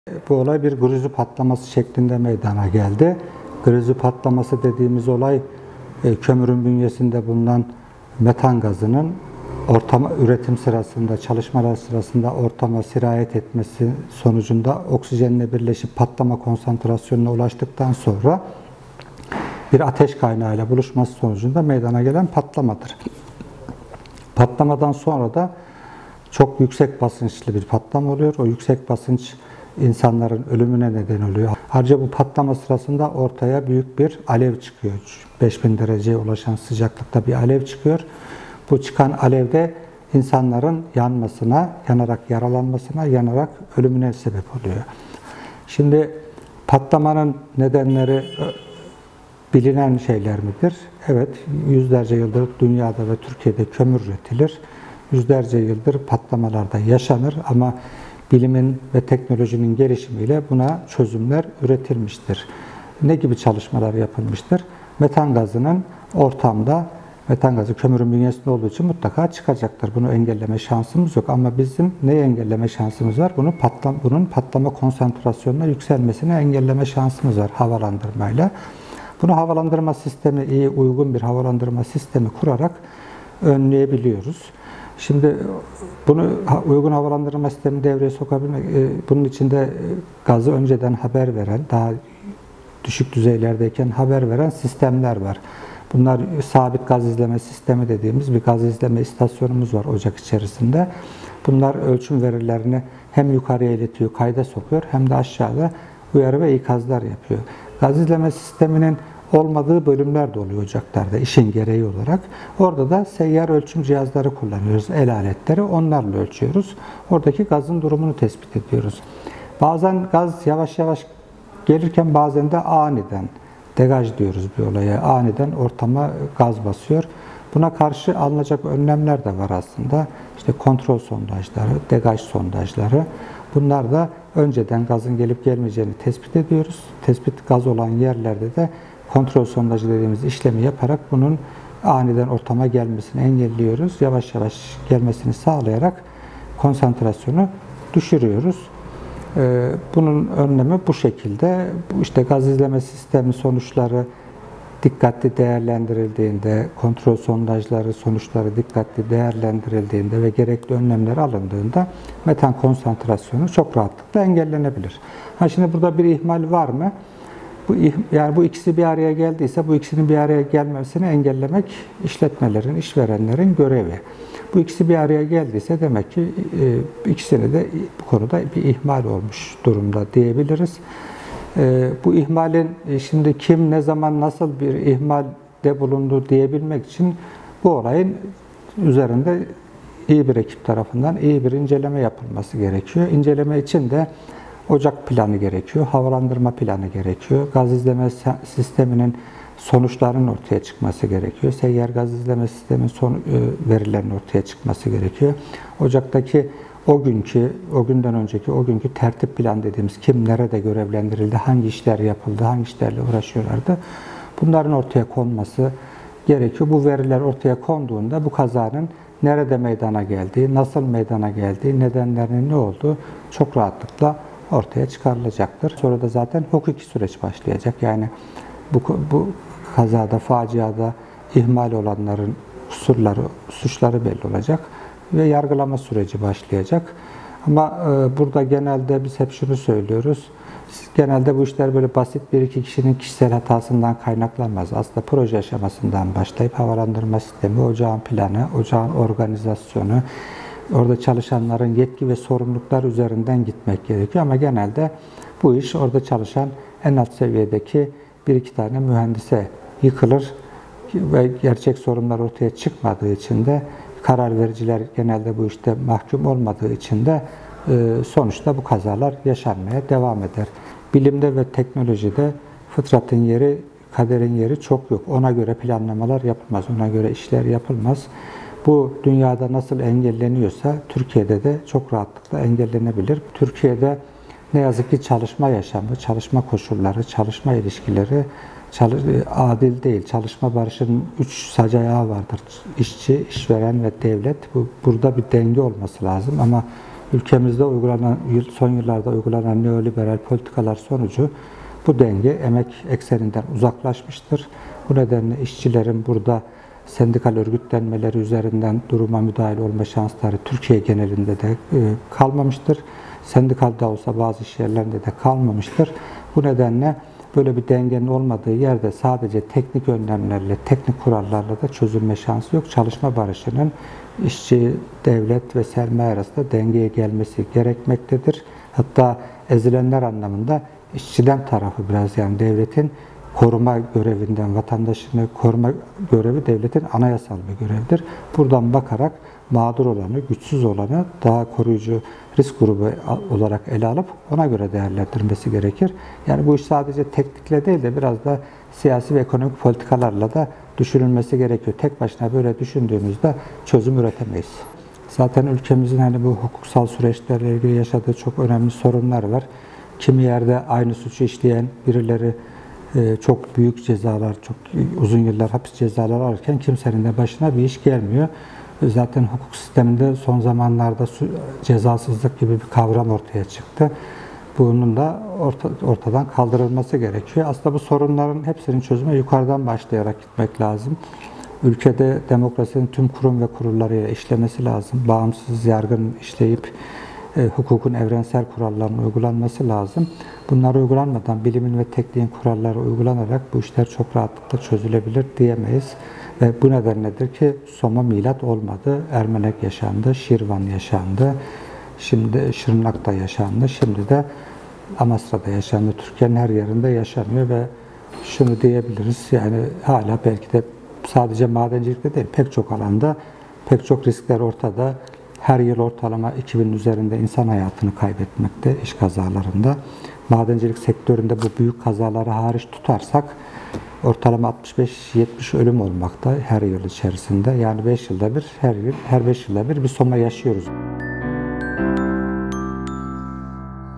AYHAN YÜKSEL: DEVLET ÖNLEM ALMADI PATLAMA ENGELLENEBİLİRDİ
MMO Yönetim Kurulu Başkanı Ayhan Yüksel, devletin önlem almadığı için Amasra’da katliam yaşandığını belirterek, “Gaz izleme istasyonları dikkatli incelendiğinde, kontrol sondajları dikkatli değerlendirildiğinde, patlamalar engellenebilirdi” dedi.